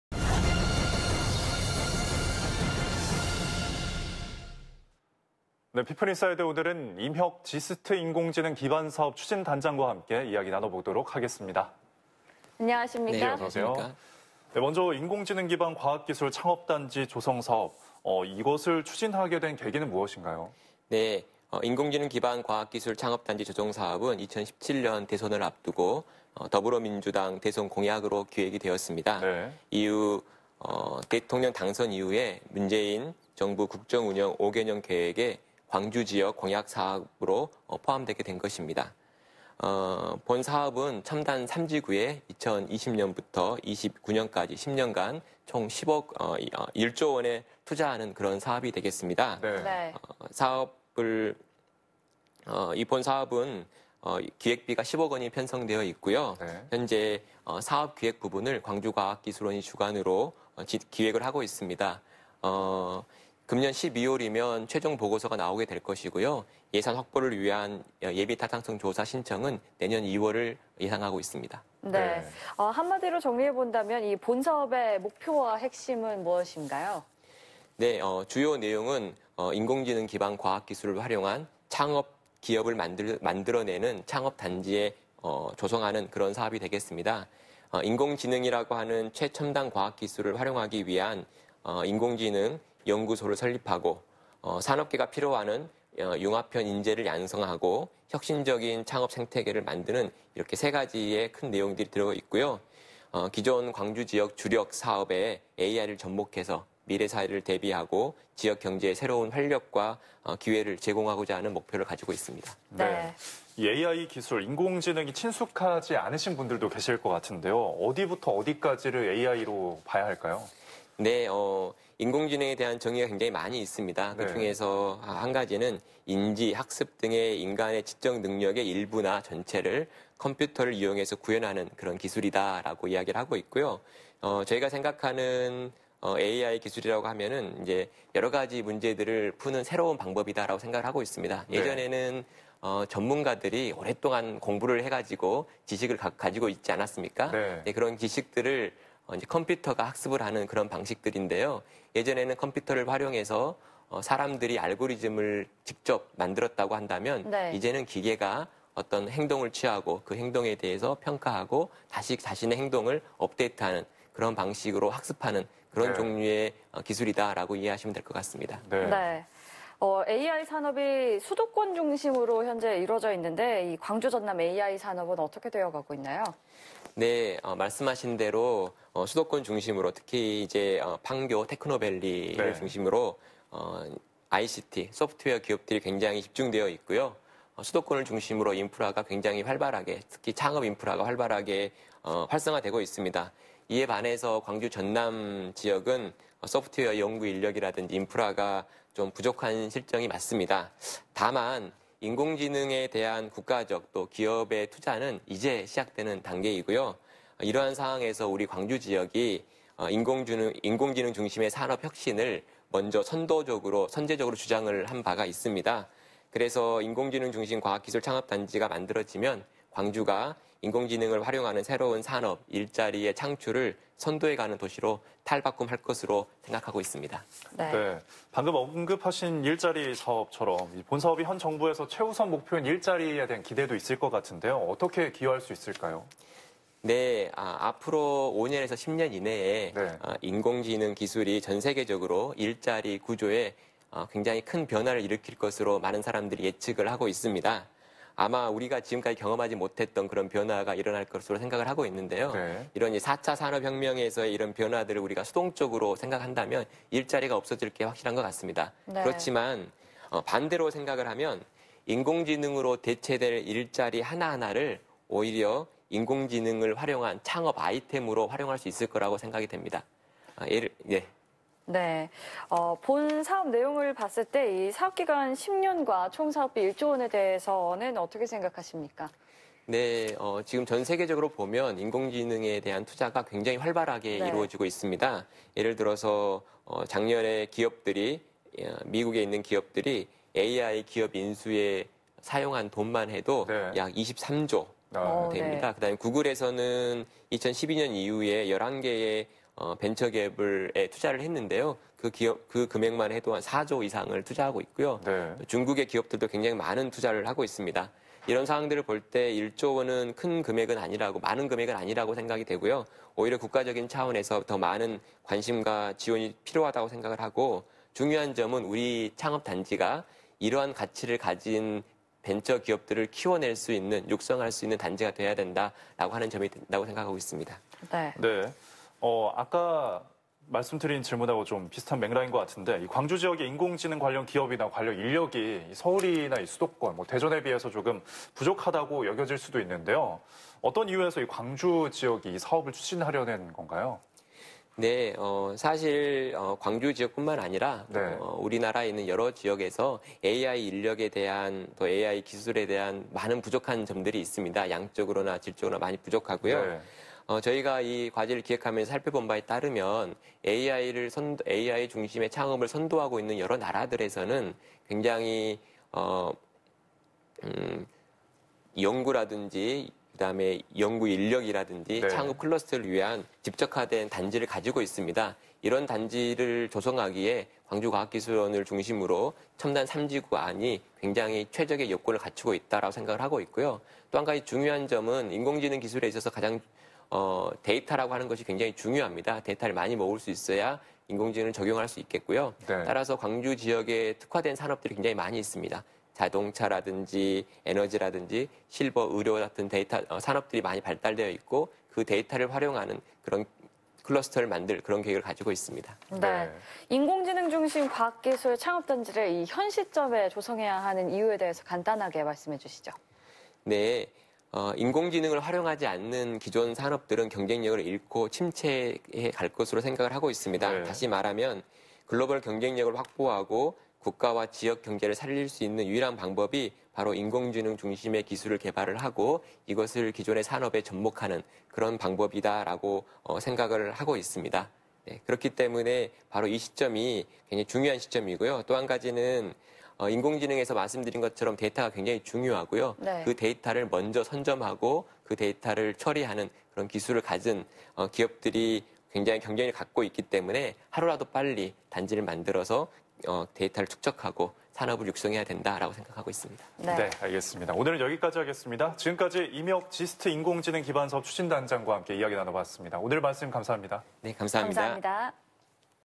임혁 GIST 인공지능 기반사업 추진단장 | 피플인사이드 20180906
피플인사이드. 오늘은 임혁 GIST 인공지능 기반사업 추진단장과 함께 이야기 나눠봅니다.

1.‘인공지능 기반 과학기술 창업 단지 조성’ 사업을 추진하는 데 광주가 왜 적합한 지역인지 그 이유는 무엇입니까?

2. 한마디로 정리해서 본 사업의 목표와 핵심은 무엇인가요?

3. AI 기술이 친숙하게 다가오지 않으실 분들도 있을 것 같습니다. 어디부터 어디까지를 AI 기술이라고 말할 수 있을까요?

4. AI 산업이 수도권 중심으로 이뤄져 있는데 광주 전남 AI 산업은 어떻게 되어가고 있나요?

5. 본 사업이 현 정부에서 최우선 목표인 일자리 창출에 어떻게 기여할 수 있나요?

6. 본 사업내용을 봤을 때, 사업 기간 10년과 총사업비 1조 원에 대해서 어떻게 생각하십니까?

7. 광주 지역에 인공지능 관련 기업이나 관련 인력이 서울이나 대전 지역 등에 비해 부족하다고 여겨지는데 어떠한 이유에서 광주지역에 이 사업을 추진하려고 하십니까?

8. 인공지능 중심 과학기술 창업 단지를 현시점에서 조성해야 하는 이유는?